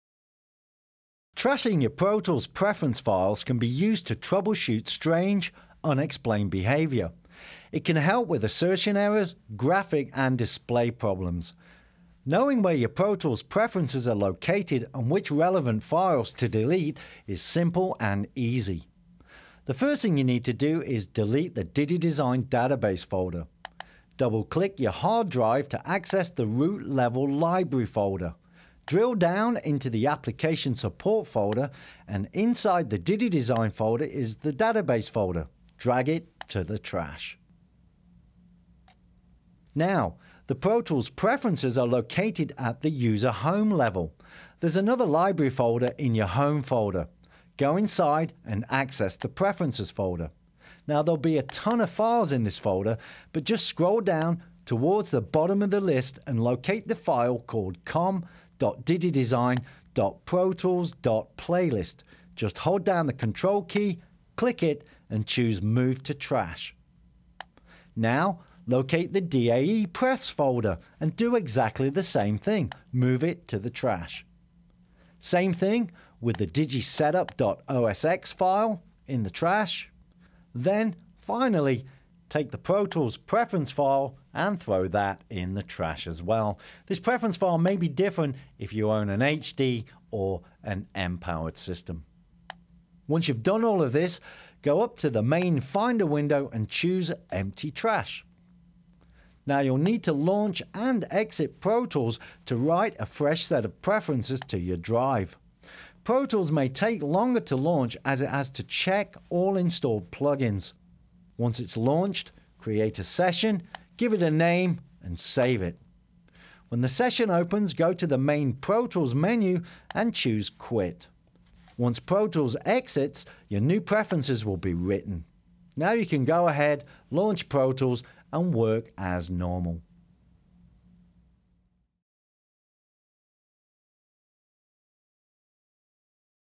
Trashing Pro Tools® Preferences - Mac OS X 10.6 - Troubleshooting Pro Tools
CLICK HERE FOR IMPORTANT TECHNICAL LINKS, AND OTHER USEFUL INFORMATION.
This movie explains how to remove or Trash, your Pro Tools Preferences completely. After this is done, Pro Tools Preferences are completely reverted to Factory Default. This includes Peripherals too.  It's important to remember that when preference files becomes corrupt, the type of strange, unexplained behavior that the application may exhibit, can be extremely frustrating. It's easy to convince yourself that this troubleshoot is not necessary. No one is immune from corrupt Preferences.

It is recommended that you take some time to make your personal changes to the Pro Tools Preferences, and the Peripheral setups, once you're back in the Application. Please watch at 720p for best picture quality.

TECHNICAL LINKS
Read the AVID Knowledge Base article for this video. KB# 349739

Optimizations relating to Mac OS X 10.5 - KB# 349731